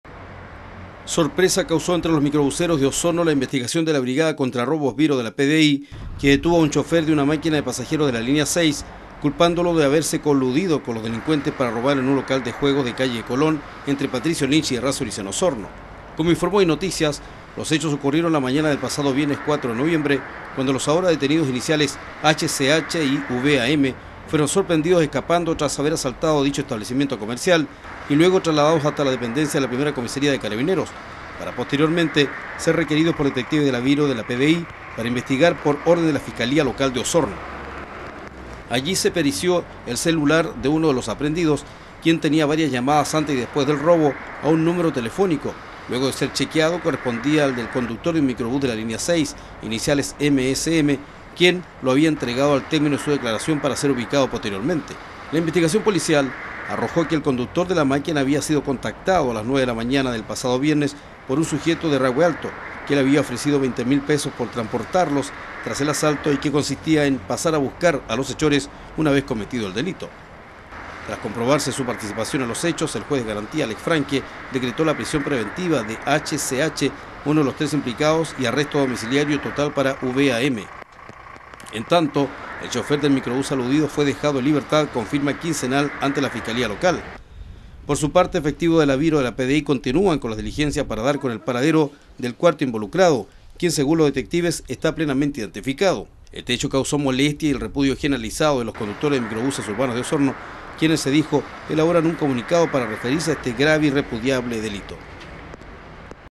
Chofer involucrado en robo
Un conductor de microbús urbano fue descubierto apoyando a sujetos que asaltaron un local de juegos de calle Colón en Osorno. Quedó con medidas cautelares.

el sitio oficial de Inet Televisión.